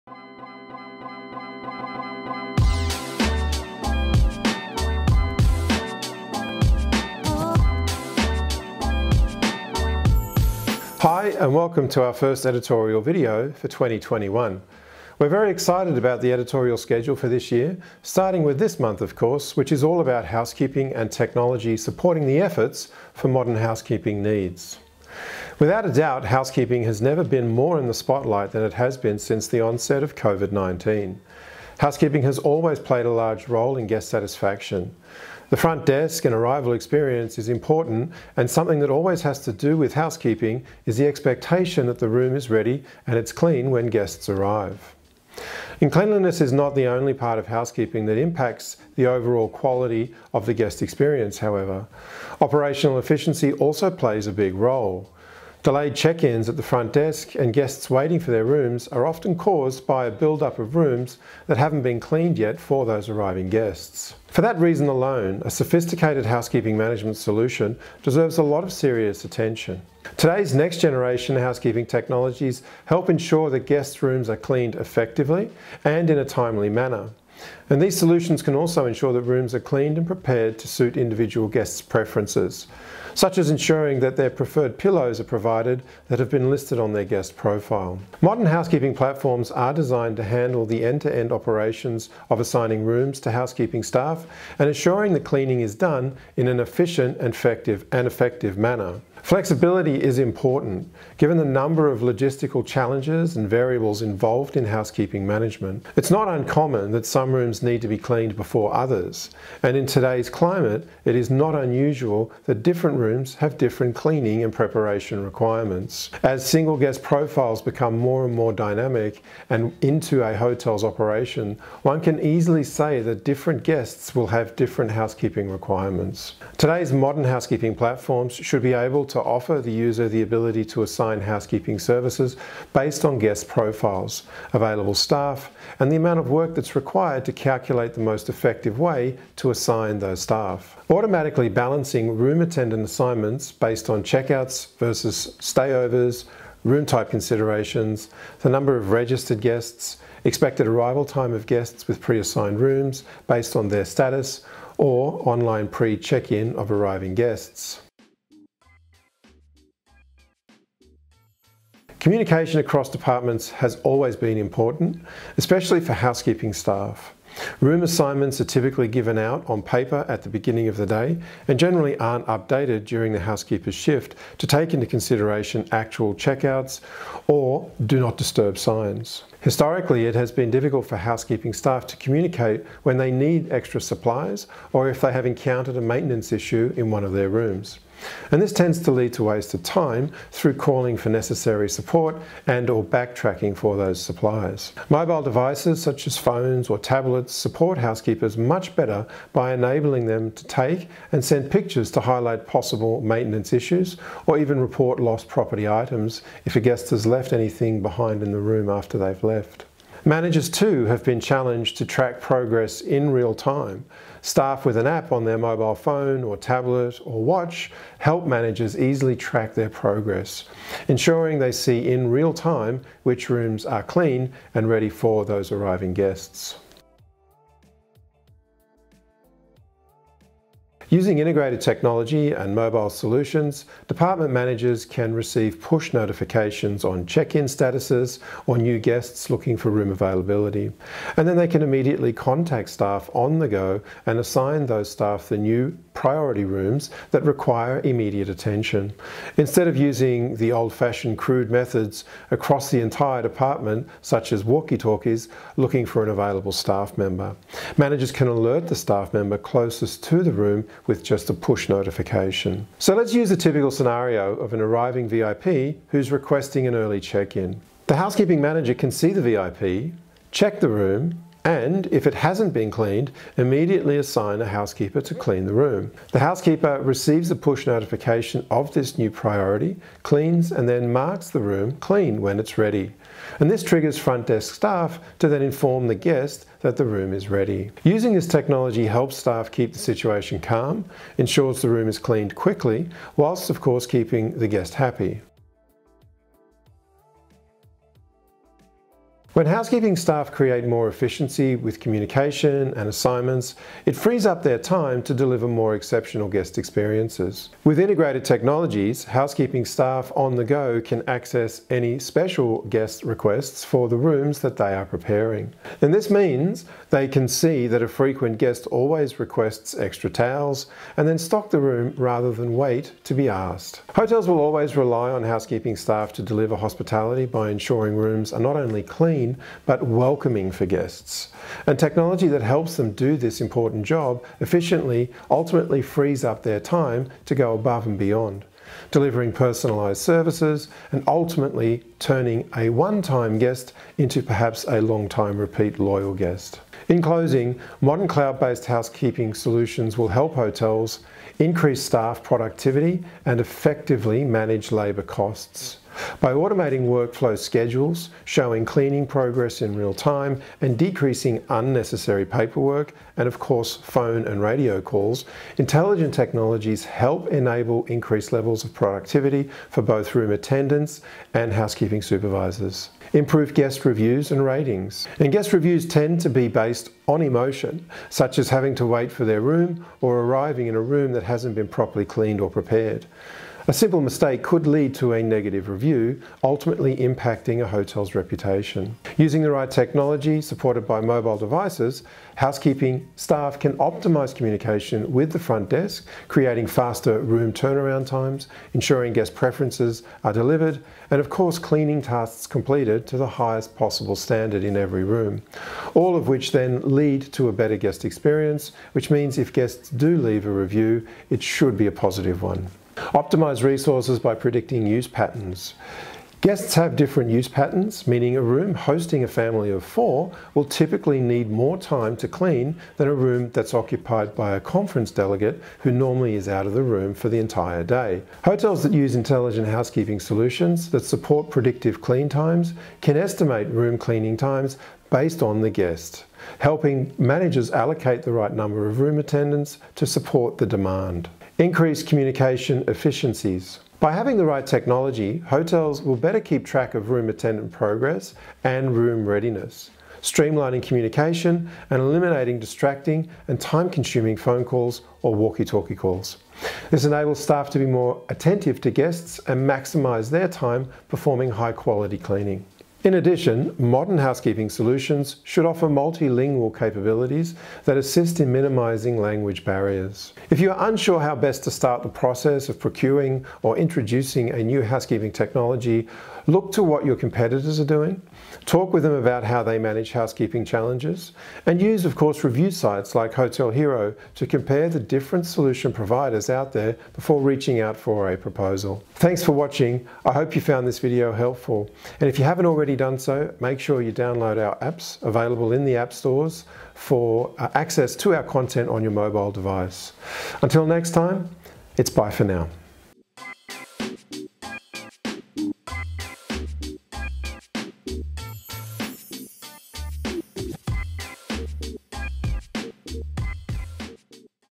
Housekeeping solutions for hotels.
This video provides insights into how modern cloud-based housekeeping solutions can support hotels' operations, ultimately increasing guest satisfaction, as well as improving operational efficiency. This video will answer questions such as, what aspects and criteria should be considered when selecting the right housekeeping technology? And, what are the advantages and benefits of housekeeping software solutions?